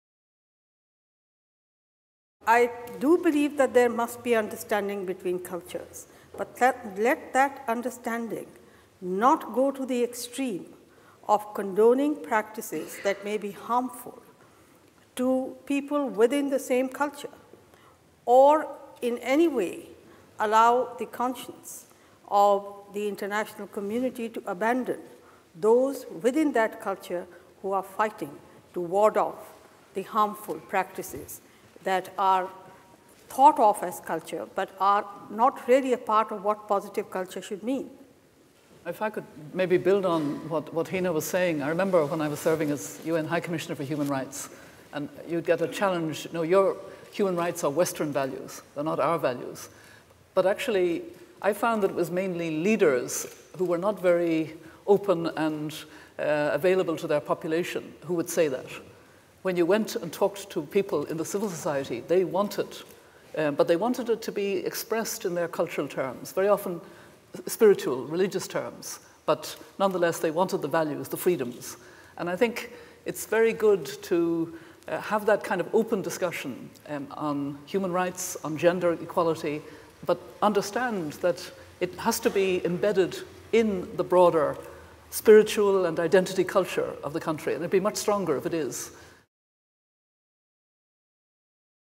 Hina Jilani: 'cultural understanding' does not mean condoning harmful practices
How should the international community promote universal human rights, while respecting cultural differences?

Hina Jilani and Mary Robinson discussed this question during the Elders debate in Oxford last week